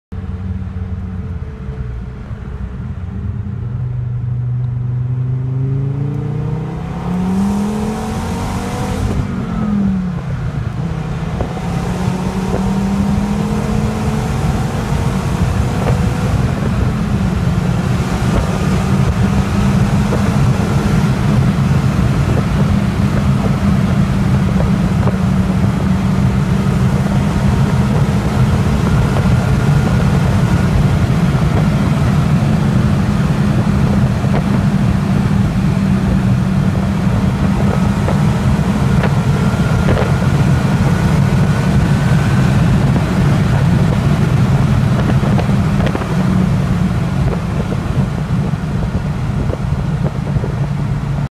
Volvo S70 T5 -00 (60 till ~200 km/h)
Provkörning hem från jobbet på en kullig riksväg mitt i natten. Utspelar sig någon gång mellan juni och september år 2017. Svårt som fanken med växlandet med endast en hand riktigt fri och den gör man ju onekligen bäst med att hålla fast i ratten ordentligt hårt, så jag känner mig alltid lika dum som ger mig upp i hastigheter på över 200 km/h och samtidigt filmar det på egen hand med min mobiltelefon... och ändå gjorde jag det i går igen med min Landsvägsslukare. :-D Ingen minns en fegis, och krockar jag dör jag med all säkerhet ändå = hedern i behåll! ;-)

Motor:

Programvara: BSR steg 3
Avgassystem: 3" downpipe, 3", 100 cellsracekatalysator, 3" från katalysator och bakåt med en ljuddämpare. (Ett 3"-system satt på redan när jag köpte den, men jag har byggt om det helt och hållet).
Luftfilter: Öppet med 3" anslutning
Magnetventil: ATM
Kortväxlingsadapter
Uppskattad effekt: 320 hästkrafter och 450 newtonmeter